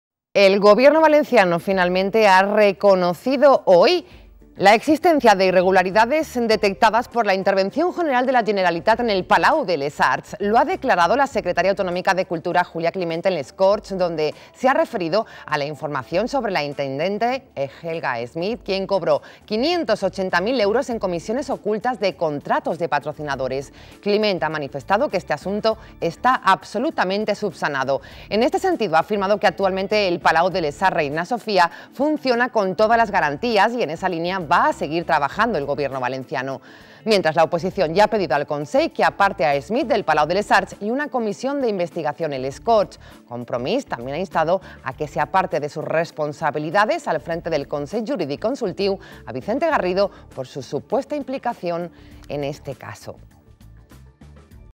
El Consell admite irregularidades en el Palau de les Arts
La Secretaria Autonómica de Cultura, Julia Climent, ha declarado que la la intendente del Palau de Les Arts, Helga Schmidt, cobró "508.000 euros en comisiones ocultas" de contratos de patrocinadores.